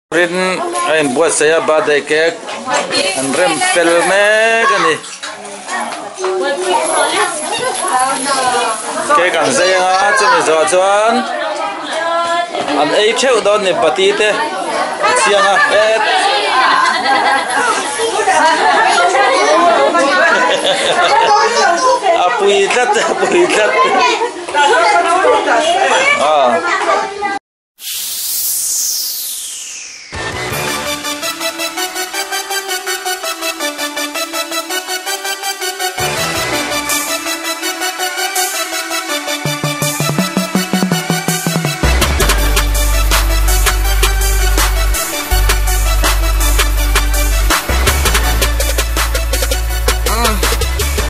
Mizo Naupang Cake Eiru..
Kan thenawm naupang hi tha mut mut a nia...